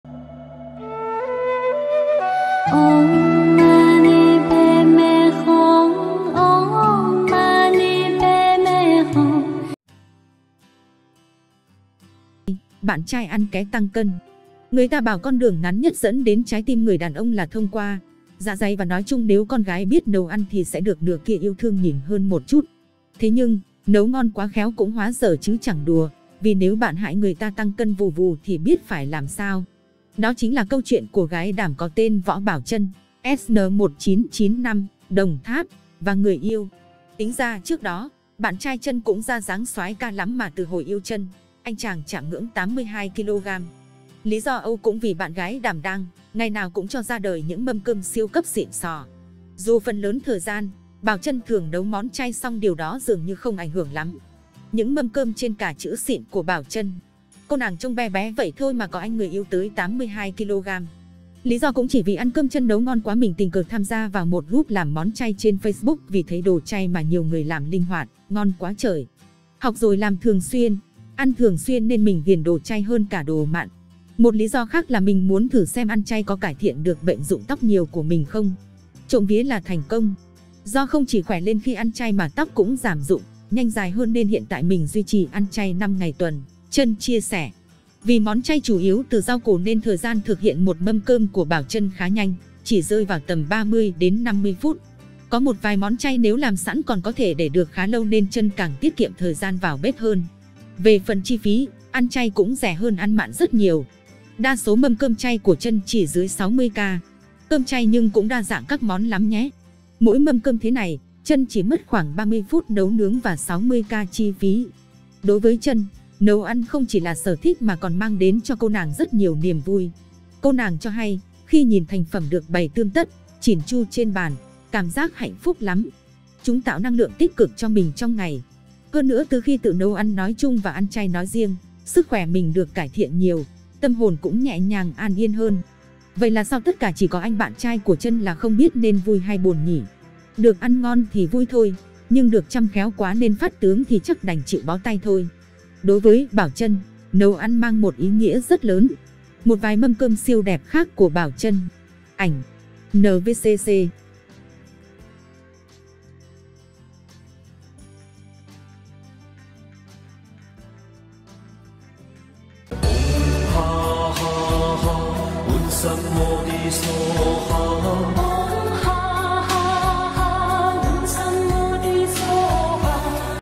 Mâm Cơm chay đa sắc màu của cô gái xinh đẹp Đồng Tháp
Gái xinh Đồng Tháp mê nấu đồ chay, bạn trai ăn ké tăng cân vù vù chạm ngưỡng 82kg
 Có một cô bạn gái giỏi nấu ăn thì không biết các anh chàng dễ tăng cân nên vui hay buồn nữa.
Du học sinh Australia tự làm xuất cơm chay nhiều màu sắc chỉ chỉ 3 AUD (40.000 VNĐ)